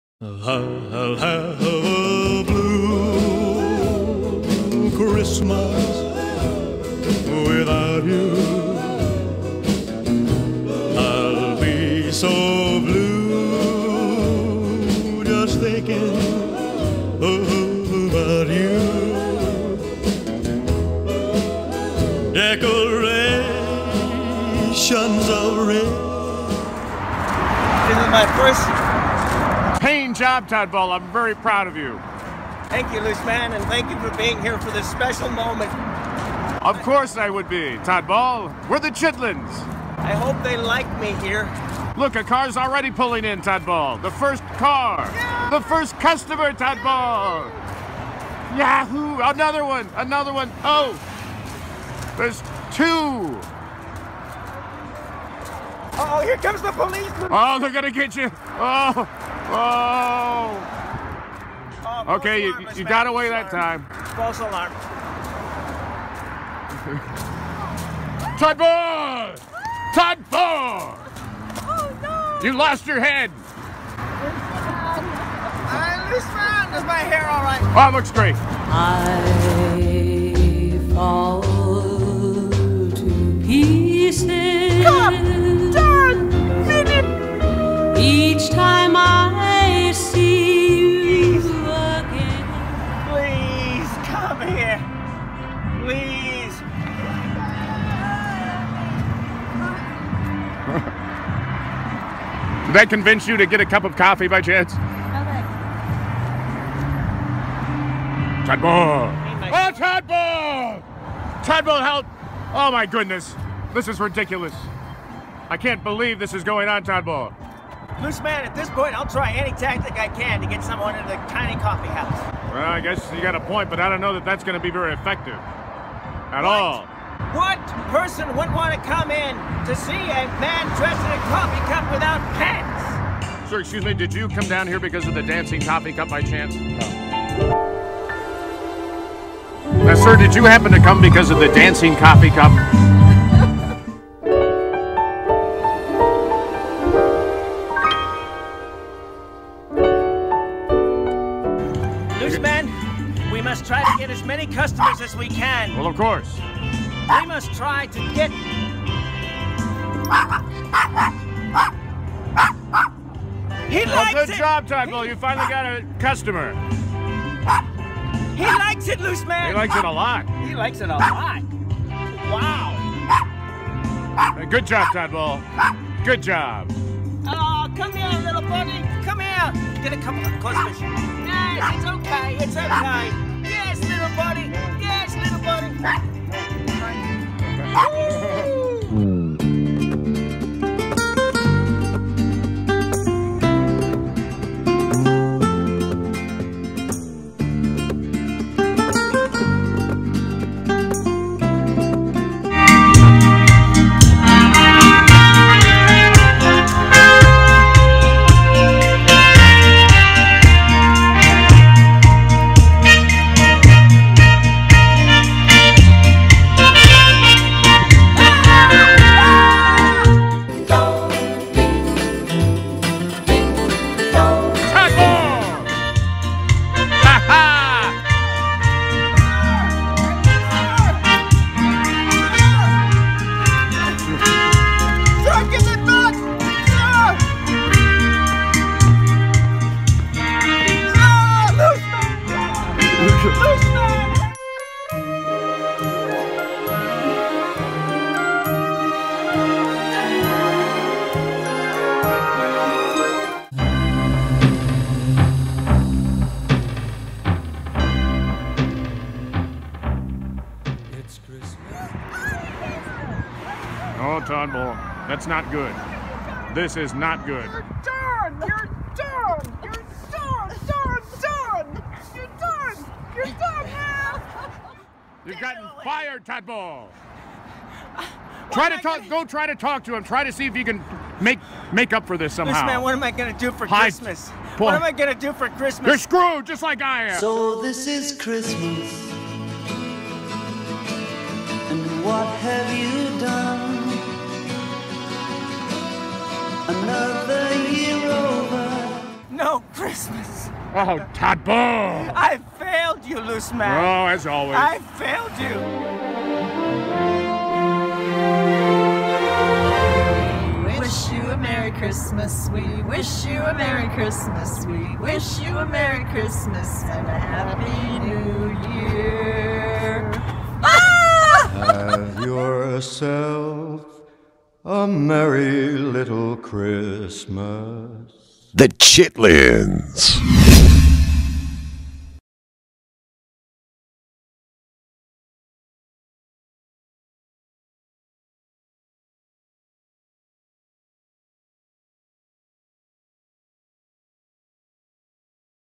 Chitlins New Coffee Christmas Job
The Chitlins are excited about Todd Ball's new job as a coffee cup. Todd Ball's job is to dress up as a coffee cup and promote The Tiny Coffee House but will the joy last?